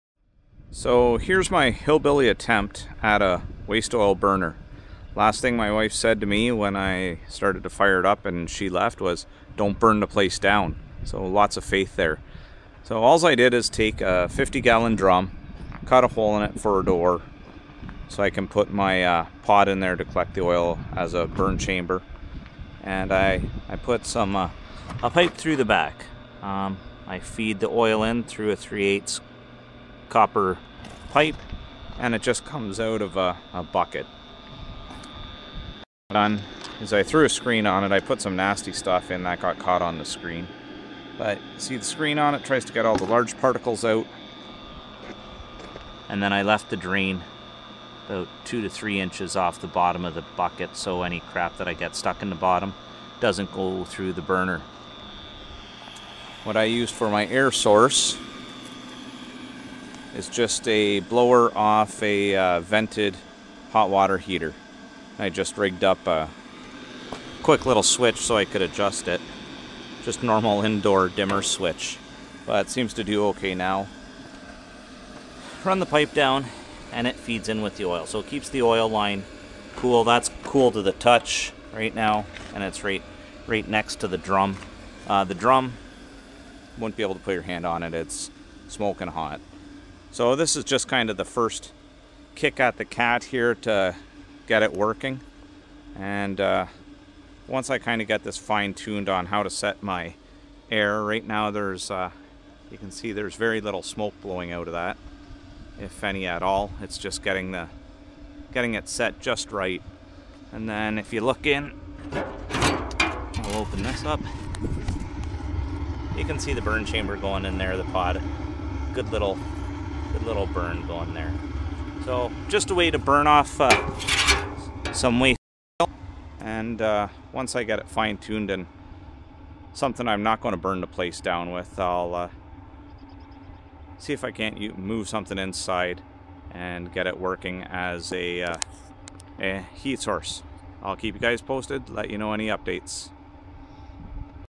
Simple Waste Oil Burner
This a quick walk around of my home made waste oil burner. This is my first attempt. I'm hoping to iron out the bugs and build better unit from an air tight stove.